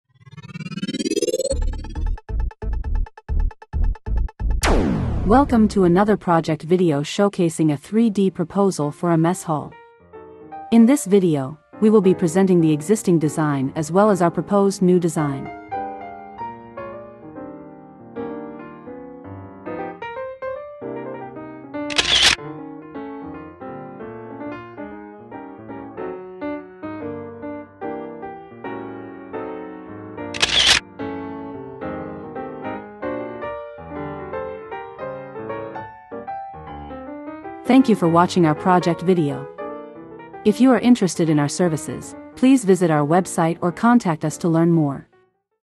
Exquisite Army Mess Hall Design | INEX Solutions
tep into the world of elegance and functionality with our meticulously designed Army Mess Hall in Rawalpindi. At INEX Solutions, we take pride in creating exceptional spaces that cater to the unique requirements of our esteemed clients. This Army Mess Hall combines sophisticated design elements with practicality, providing a welcoming ambiance for our armed forces personnel. Discover the seamless blend of aesthetics, functionality, and comfort in this remarkable project. To learn more about our architectural and design services, visit our